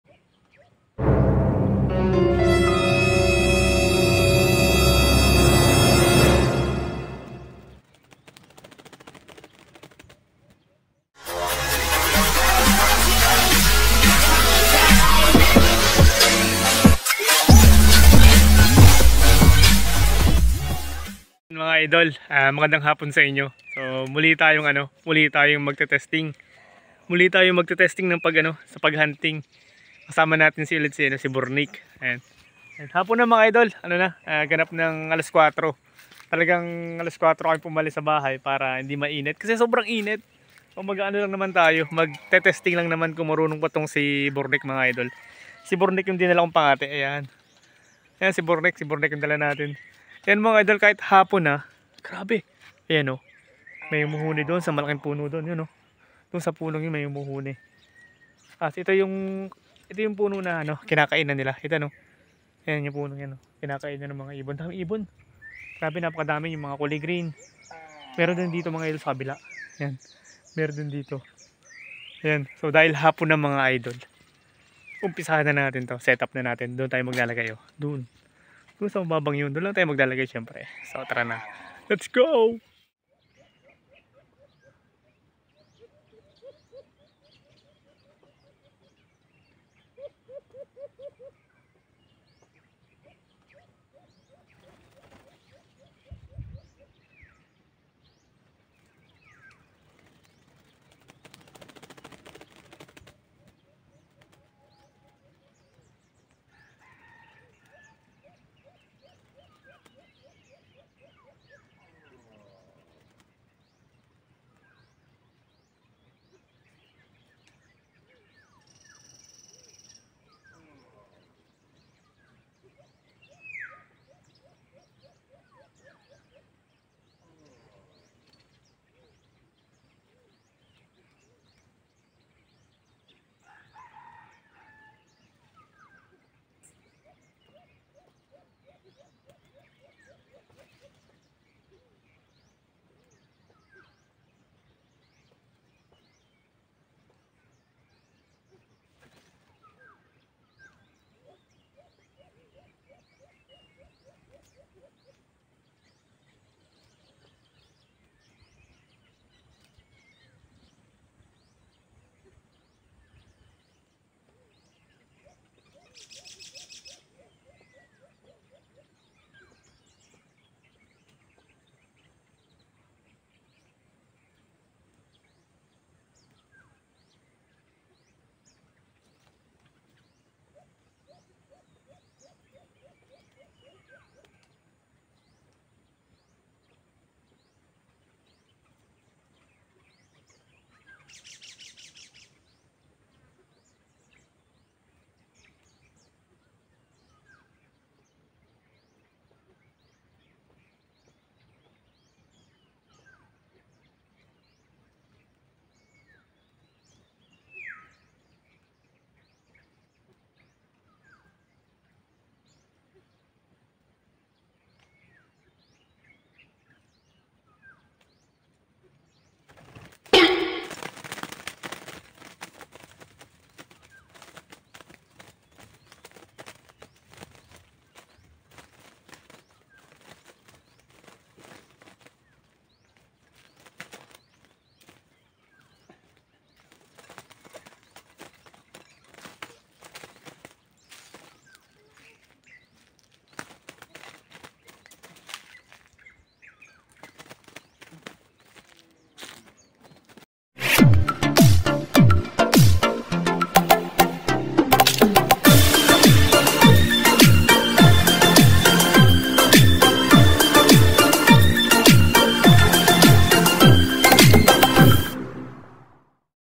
Nakaka gulat na pagbabalik napaka galing ni bornik /White Eared Brown Dove
White eared brown dove
How to catch bird
Catching bird using bird cage

Alimokon Hunting